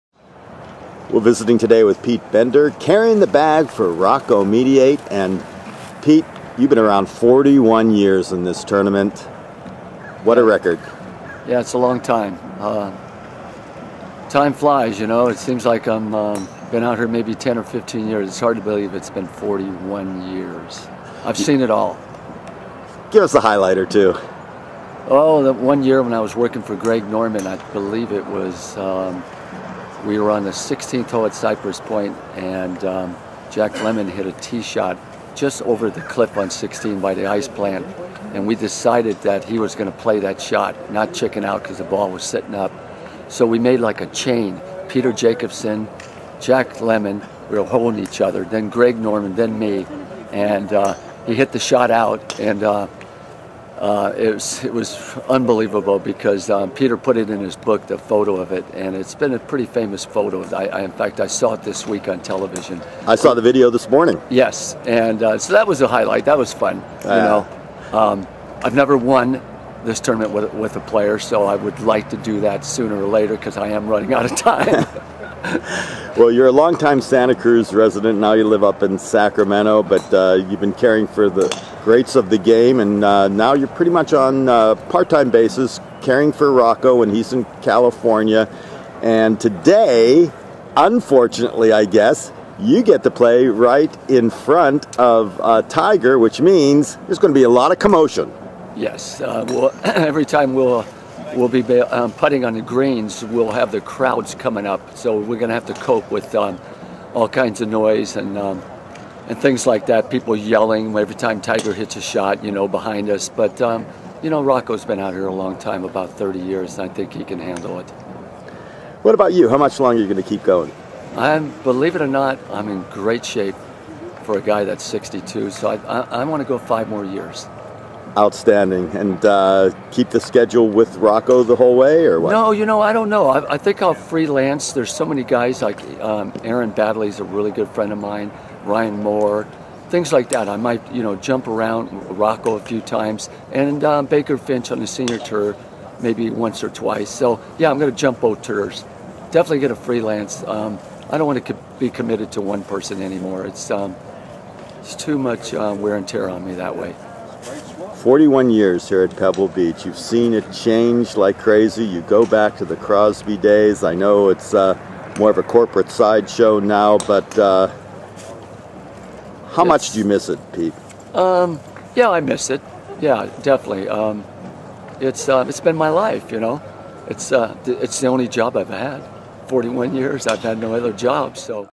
Pete Bender has caddied at the Crosby/AT&T at Pebble longer than any other looper.
For 30 years, carrying golf bags for pros on the PGA Tour was Pete Bender's full time job.  Now in his 60's, the former Santa Cruz resident has cut his schedule to part time, but has worked 41 consecutive Pebble Beach Pro-Ams.  This week, Rocco Mediate is his pro, but has history with Hale Irwin, Greg Norman, Ian Baker Finch and many others over the years.  He's seen how the tournament has changed from the "Crosby days" and admits it's not quite the fun it used to be...but it's what he does.  As a professional caddie Bender knows the nuances of what it takes to win, but so far, has never visited the winners circle with his pro at Pebble Beach.  He has however won a "major" with Greg Norman, and numerous other tour stops over the years, but Pebble Beach is his home track and time is running out to capture that elusive victory here.